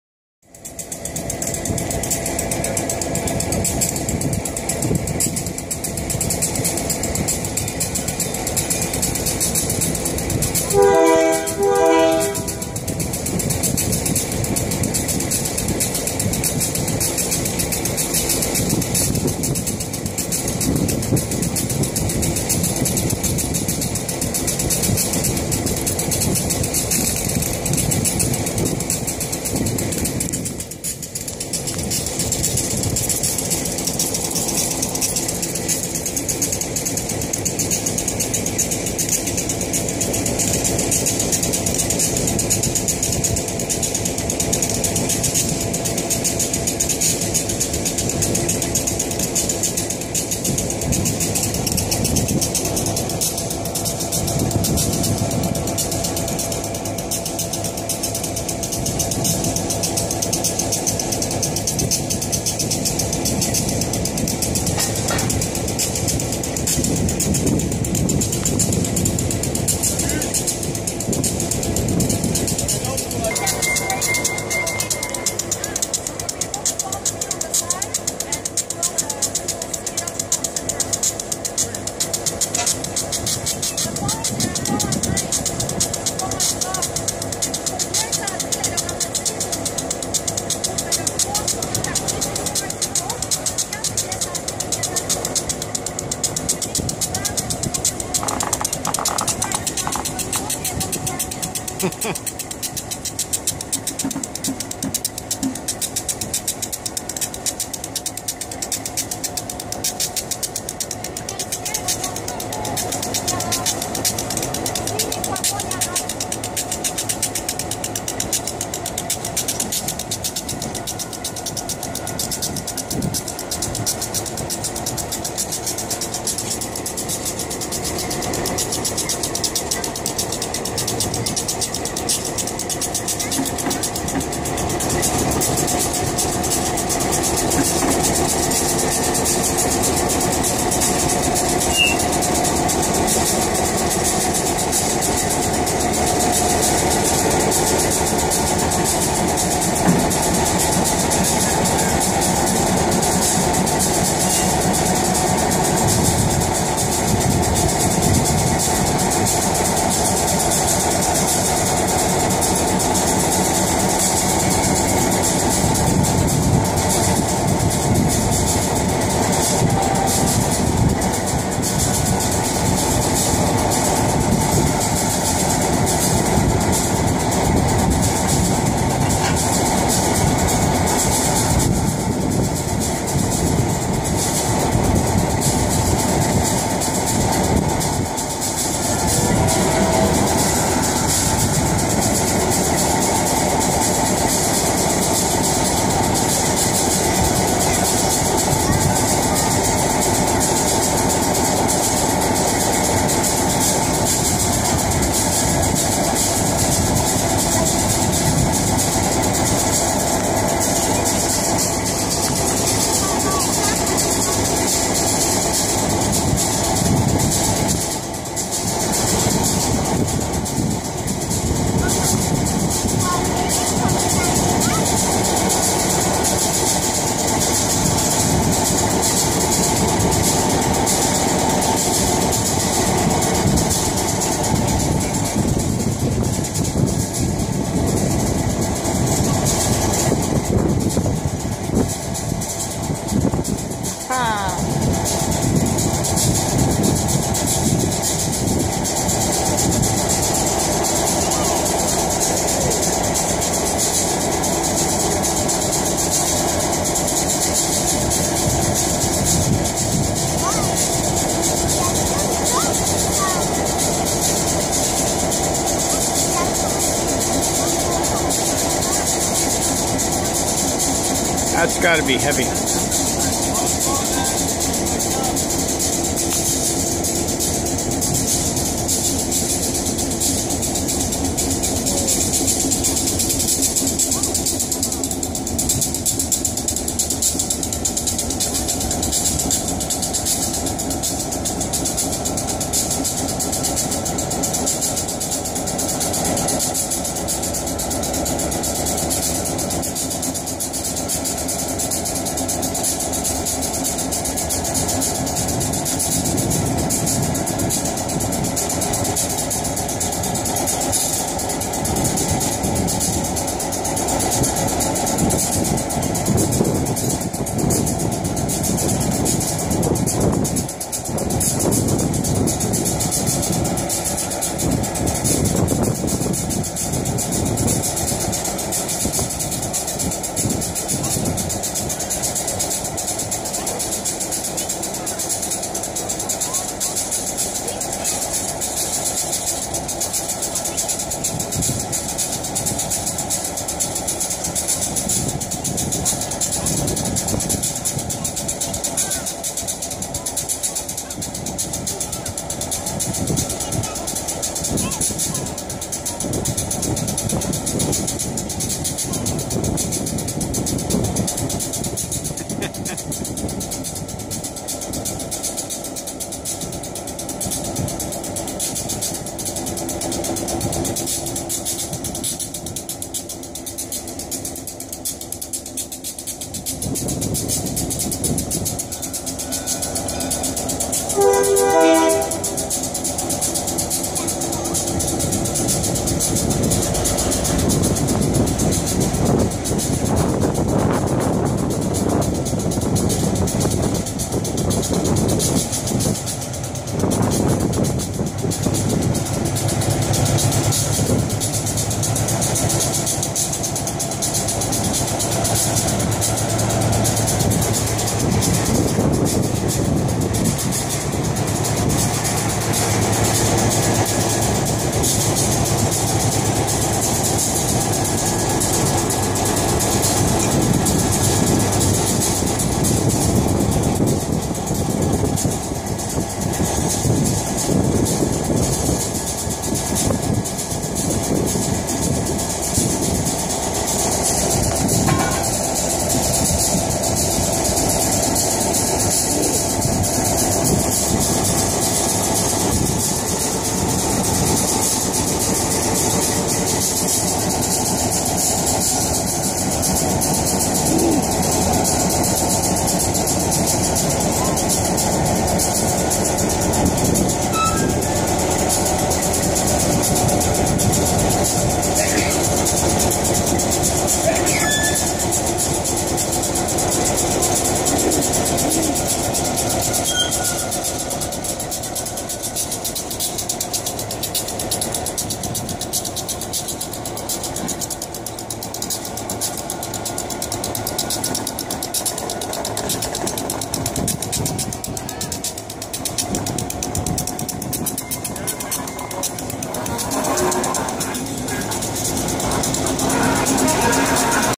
Exporail Turntable Sep 13, 2015
Turntable in operation at Exporail on Diesel Days, September 13, 2015.
The Canadian Railway Museum, aka Exporail is a great place for rail fans.